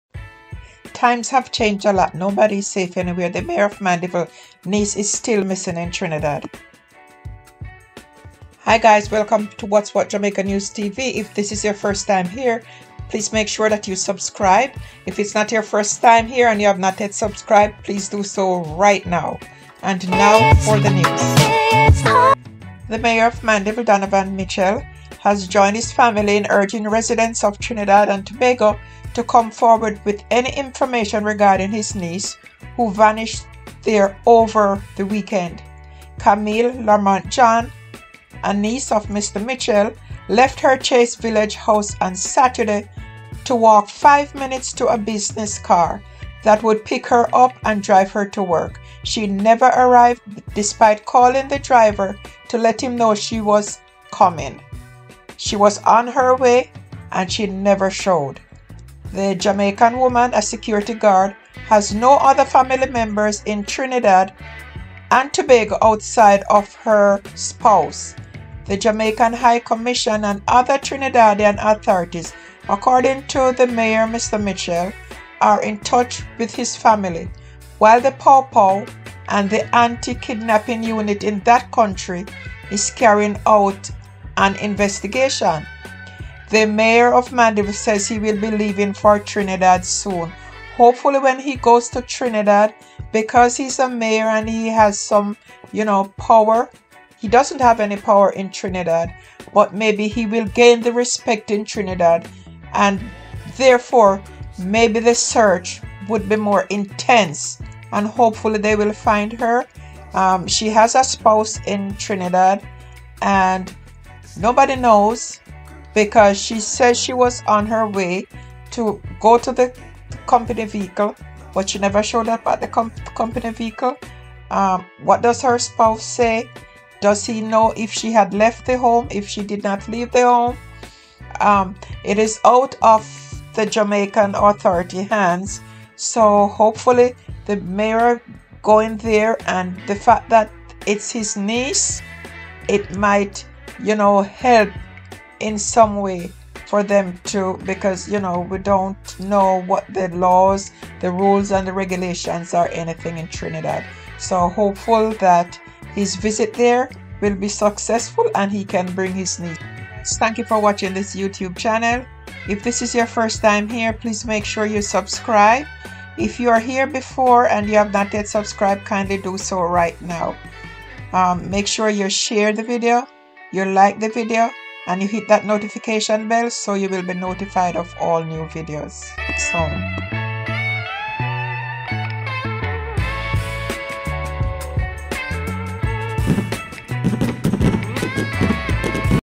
JAMAICA NEWS| Mayor of Mandeville Niece Kamalee Larmont John Still Missing in Trinidad#Jamaica
Jamaica News Today. Mayor of Mandeville Niece Kamalee Larmont John Still Missing in Trinidad